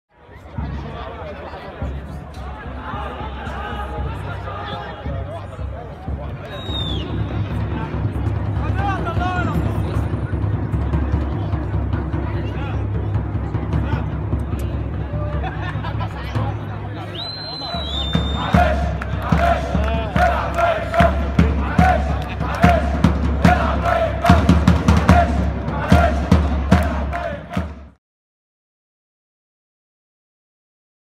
ظهور عمر جابر بمباراة فريقه مع الداخلية بعد استبعاده بسبب الإصابة
تصوير: محمد العزالي، عبدالرحمن جمال